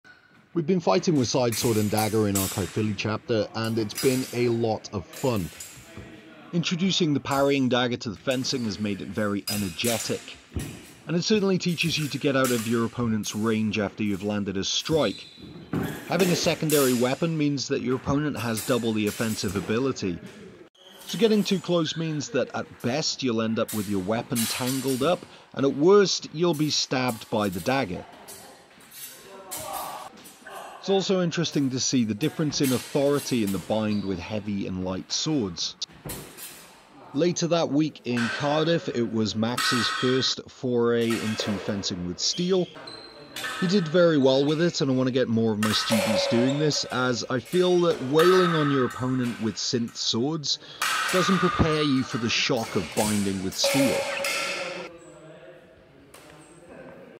sidesword and dagger fun and sword and buckler bust-up!
some footage of my sparring over the past week.